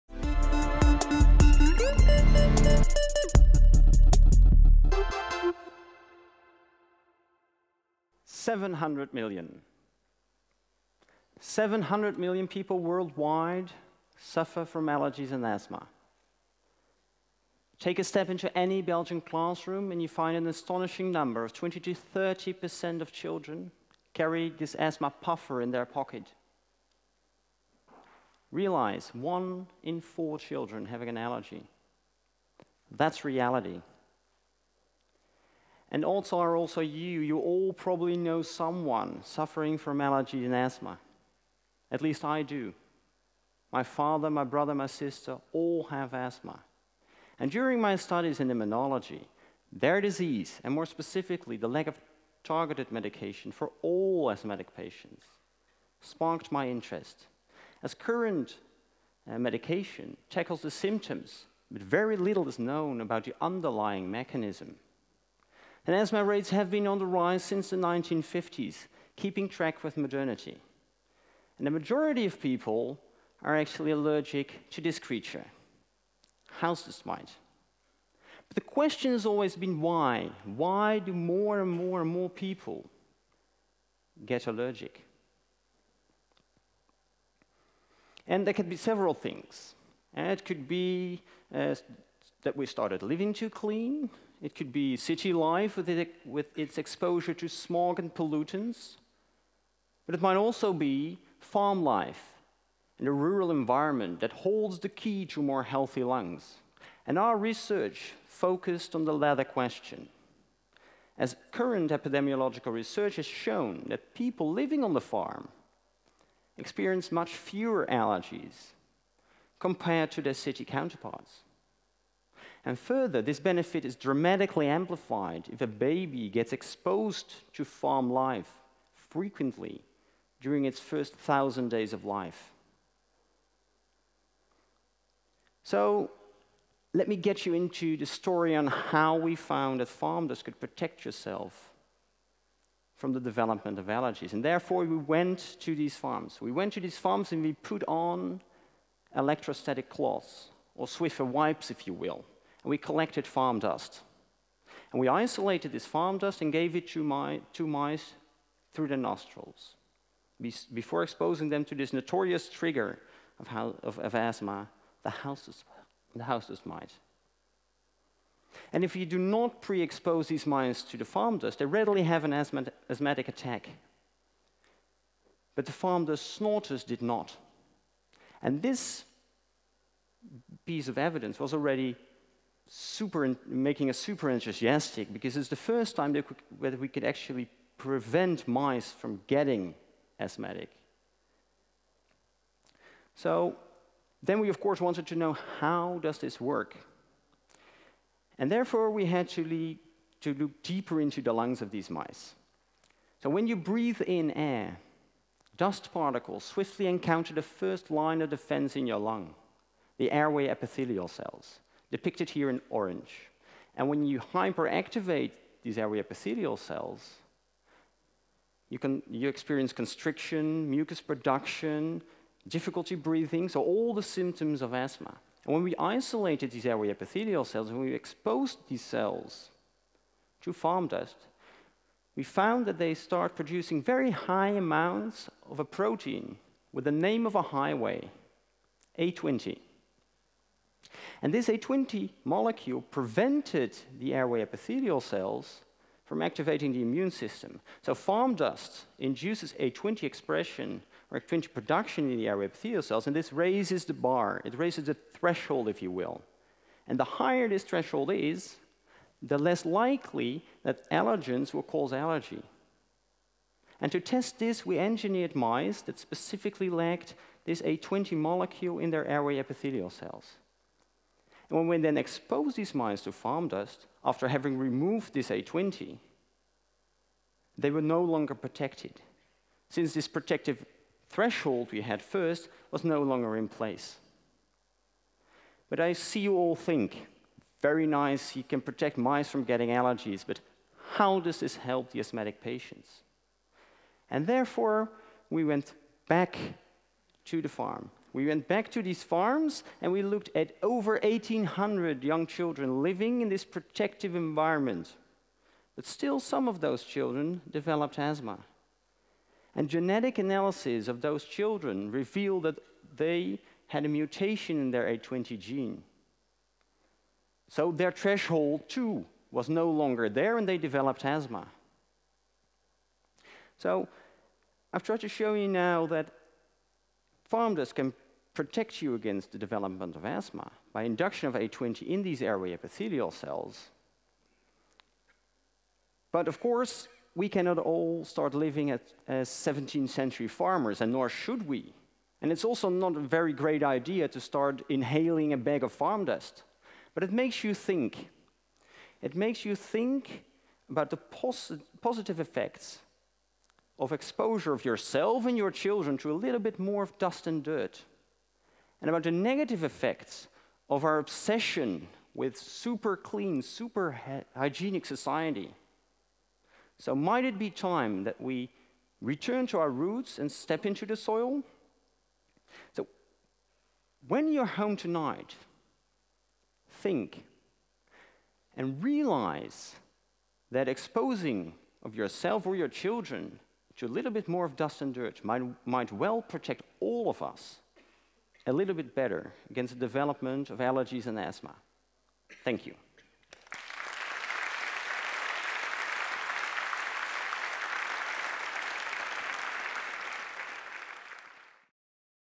From Barn to Bedside - How Farm Dust protects from Allergies | Martijn Schuijs | TEDxGhent
Asthma is an ever increasing health problem affecting large parts of the Belgian population. But why are so many people allergic and is there anything we can do? Join Martijn Schuijs on a scientific journey into the world of allergies, the consequences of our super hygienic way of living and discover which role farm dust plays in his story.

Martijn is the winner of our annual TEDxGhent PhD/post-doc Contest! He is a 5th year PhD student in Biomedical Sciences at Ghent University, the Inflammation Research Center and the Laboratory of Immuno-regulation and Mucosal Immunology.